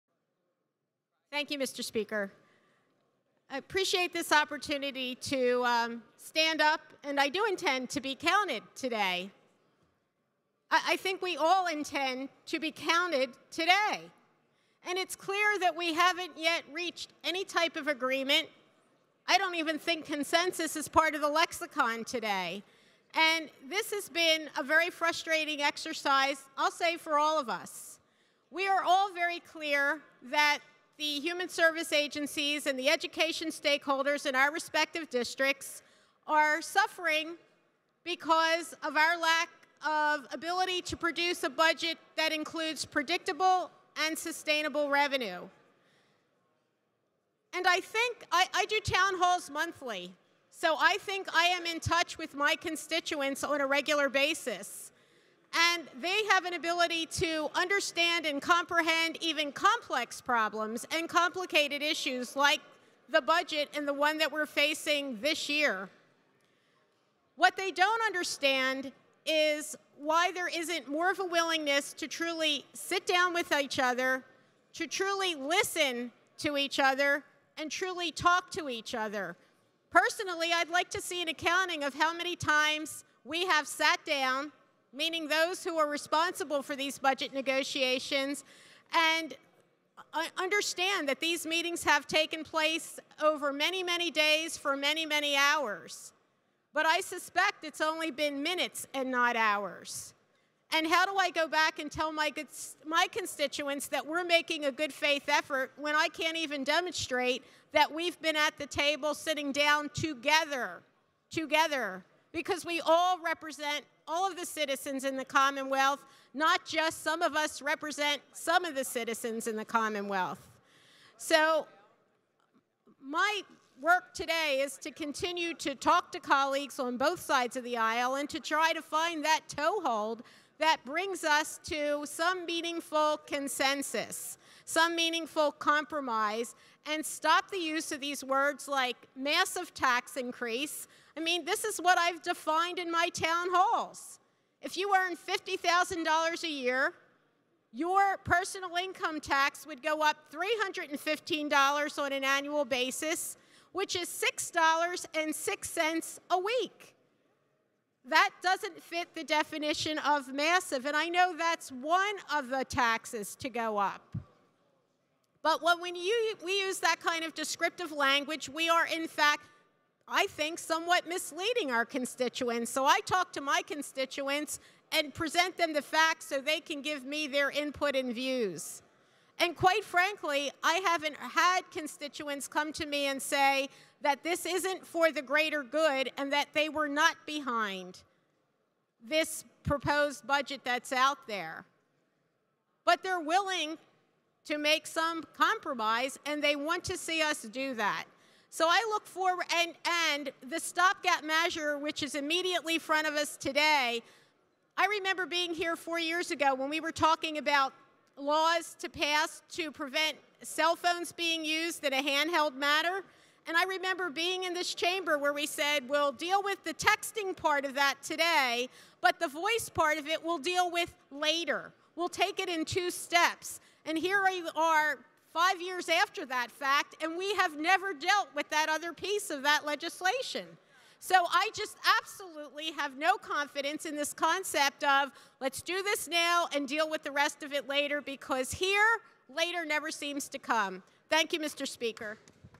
DeLissio: We Need a REAL Budget
Pa. state Rep. Pam DeLissio speaks out against a short-term stopgap budget that does nothing more than create another budget crisis for schools and non-profit social service agencies until October and urges the passage of a true compromise budget that delivers long-term, sustainable funding.